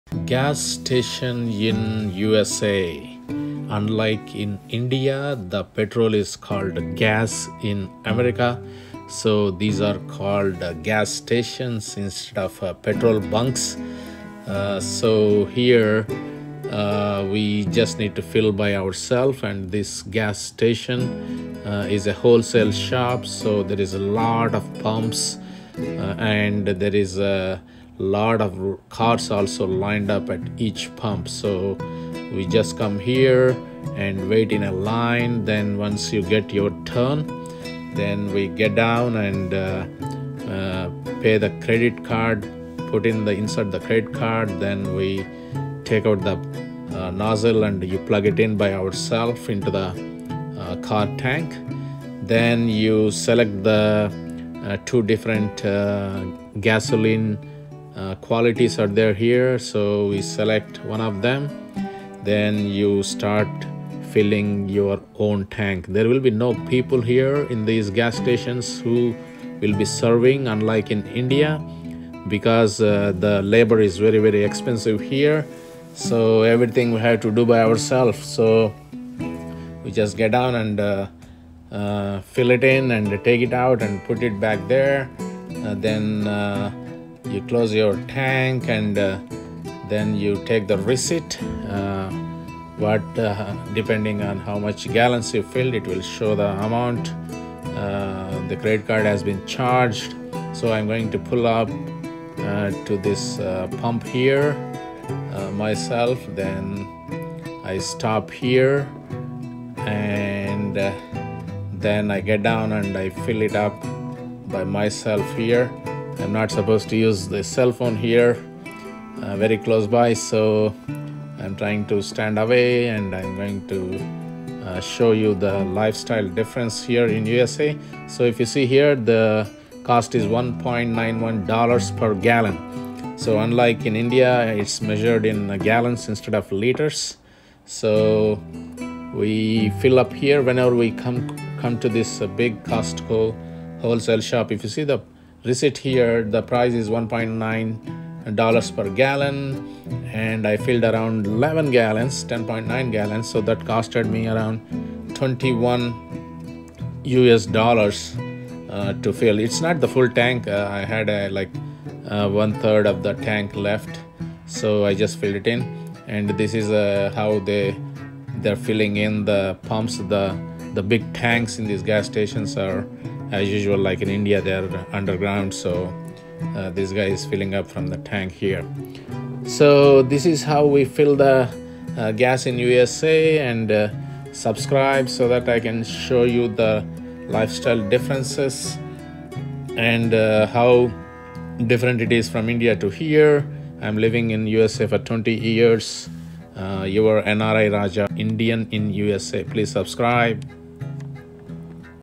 How USA PETROL Bunk Works? | NRI Desi Vlogs in English | Indian in America
Here is TOUR of AMERICAN PETROL BUNK. Unlike in India, Petrol bunks in the USA are called GAS STATIONS. I explained in this video lifestyle of NRI's in the USA and how we fill gas by ourselves, how it's measured in gallons,  how much it costs per gallon  and how much I paid for the total tank. This is a wholesale gas station of COSTCO where the prices are cheaper compared to other gas stations.